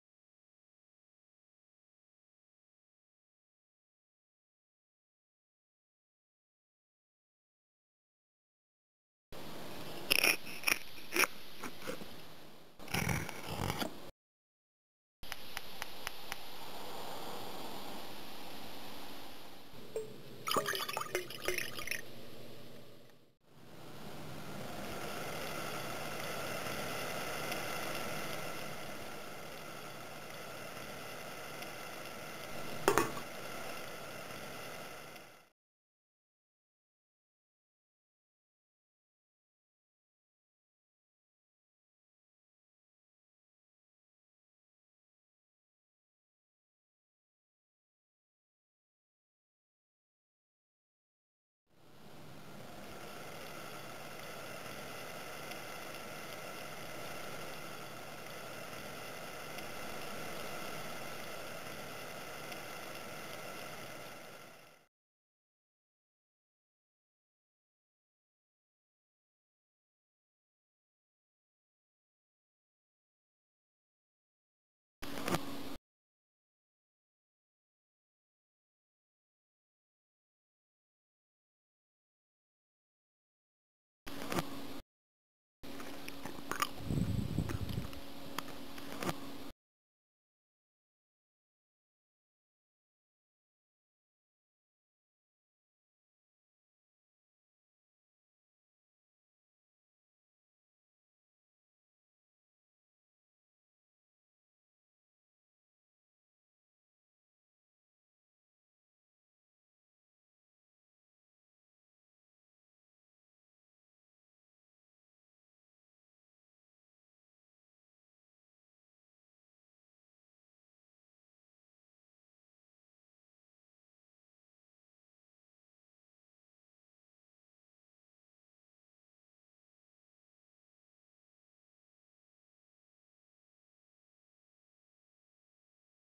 Artichoke - How to Cook & Eat
How to steam and eat an artichoke, done in the incomparable 99 Cent Chef style. And visit my cooking blog for photos & the recipe